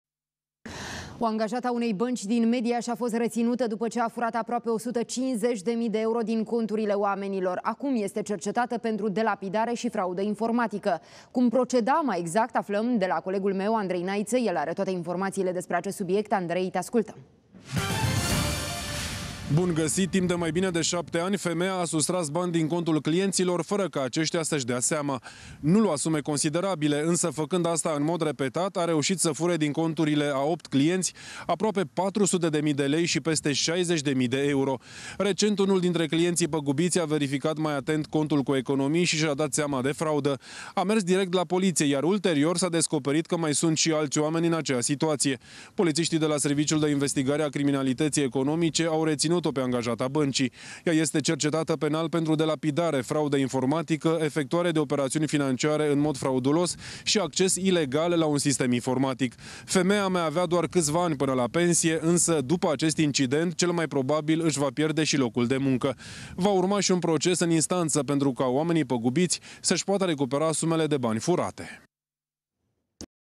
Angajata unei bănci din Mediaş a sustras din conturile clienţilor peste 140.000 de euro
O angajată a unei bănci din Mediaș a fost reținută după ce a furat peste 140.000 de euro din conturile oamenilor. Acum este cercetată pentru delapidare și fraudă informatică.